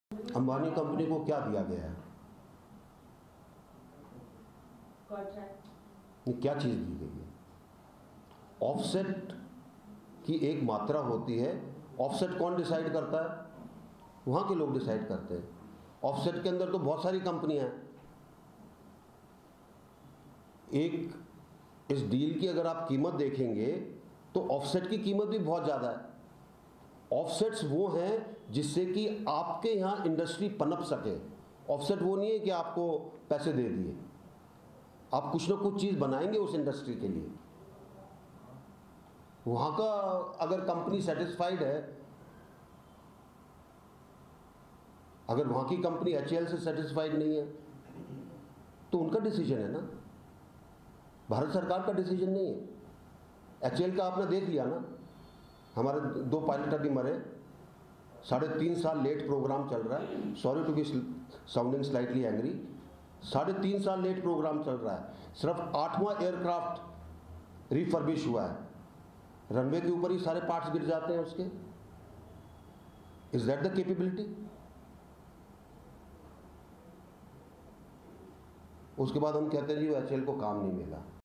VK Singh raises questions over HAL's capability
Union minister and former Army chief VK Singh has raised questions over the capability of the sate-owned Hindustan Aeronautics Limited (HAL) in refurbishing fighter planes, saying parts of aircraft had fallen off the runway, in an apparent reference to the recent Mirage 2000 crash in which two pilots were killed.